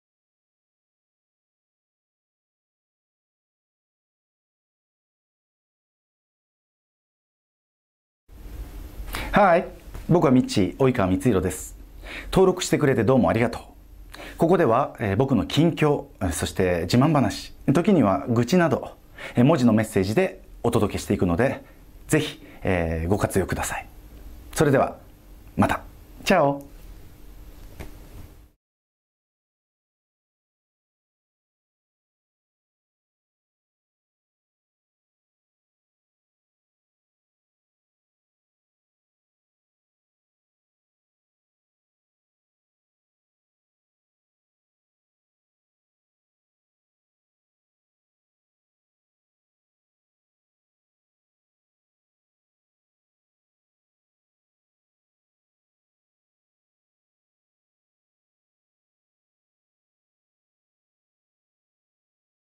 及川光博 (Mitsuhiro Oikawa) Mitchy Message me On my LINE App!!
Tiday's video i will to you guys on Sr Mitchy (Mitsuhiro Oikawa) message to me on My LINE app!! Earlier at 8:17 am Philippine time and 7:17 am Japan time in the morning August 12, 2023 hehe thank you sr Mitchy for your message 😍🥰